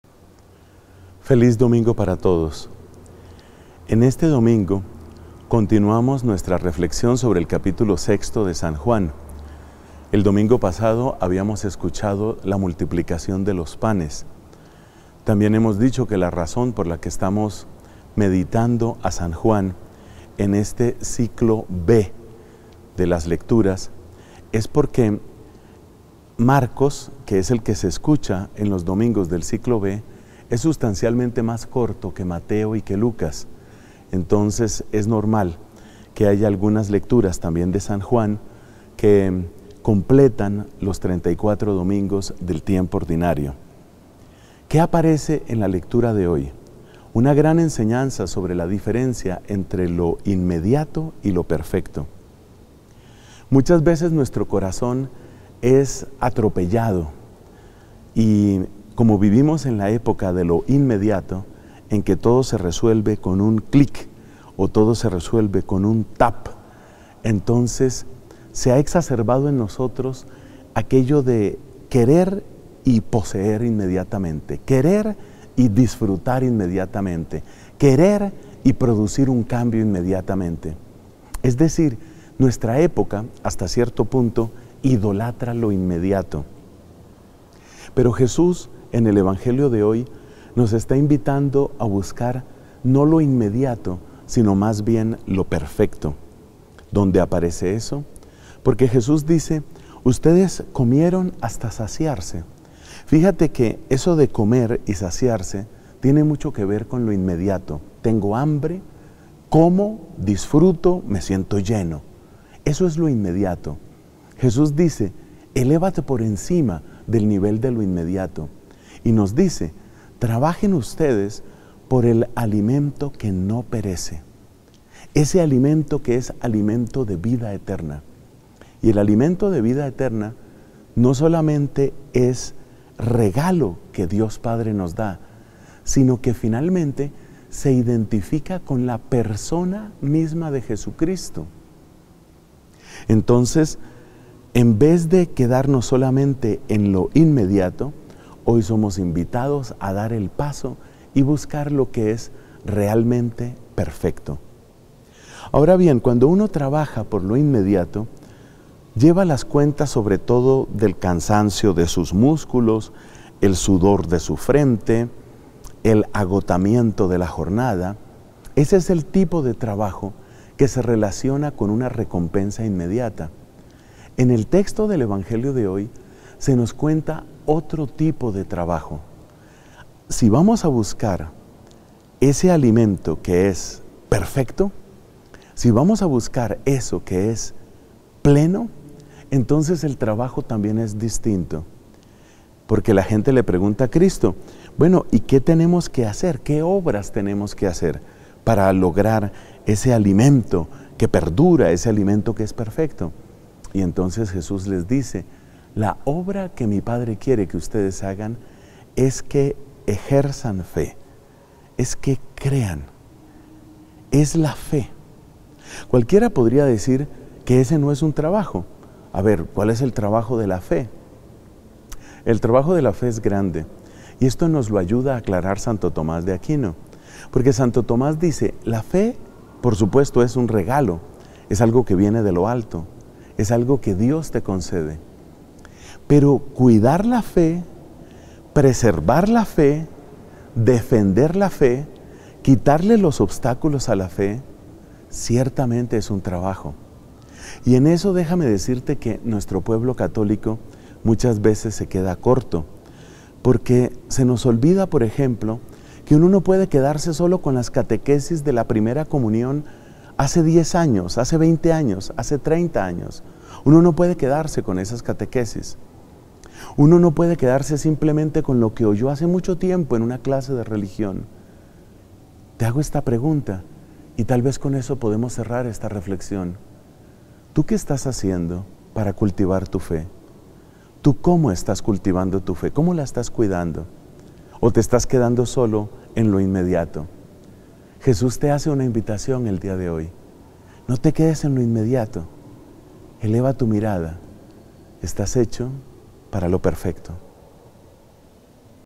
LA GRACIA del Domingo 5 de Agosto de 2018
DOMINGO XVIII DEL TIEMPO ORDINARIO, CICLO B
¿Qué estás haciendo para cultivar tu fe? Jesús te invita a no quedarte en lo inmediato, eleva tu mirada porque estás hecho para lo perfecto.